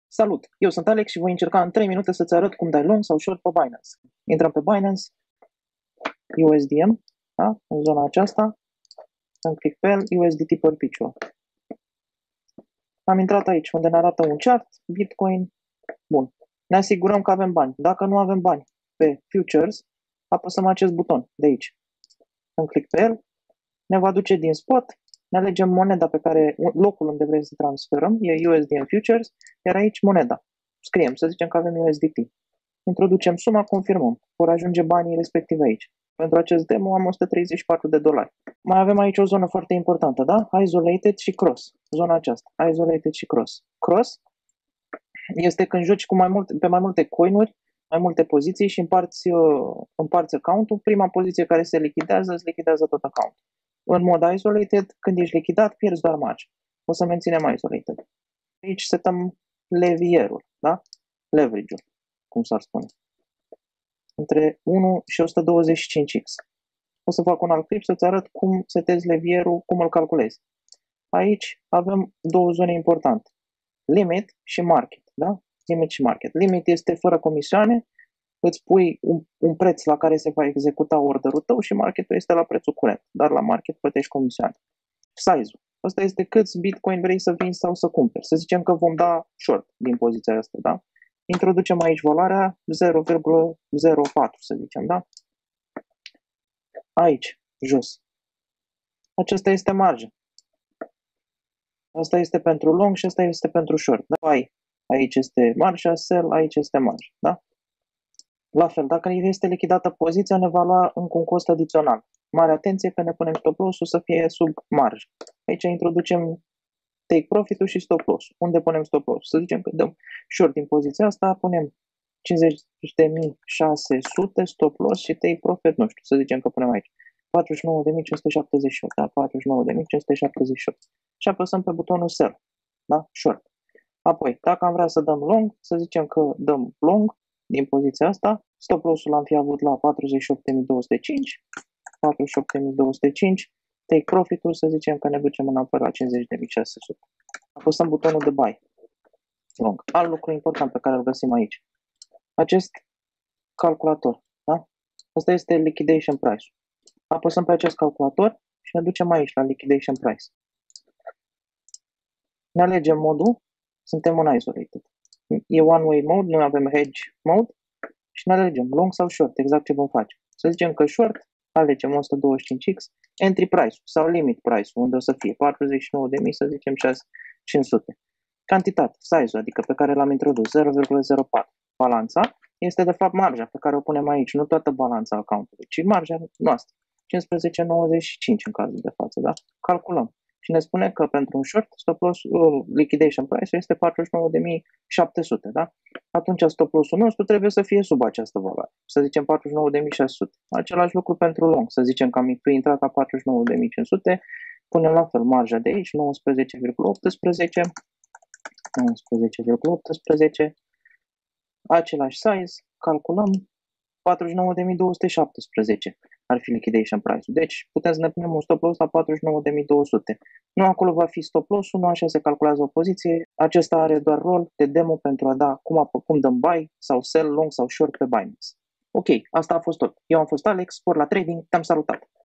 3dingschool - [Binance Futures] cum dam long/short
3ding school e un concept in care, in aproximativ 3 minute incerc sa prezint notiuni elementare de trading care sa te ajute in aventura ta spre profitabilitate!

Pe mine ma gasesti activ pe urmatoarele canale

Daca vrei sa te apuci de trading incearca FTX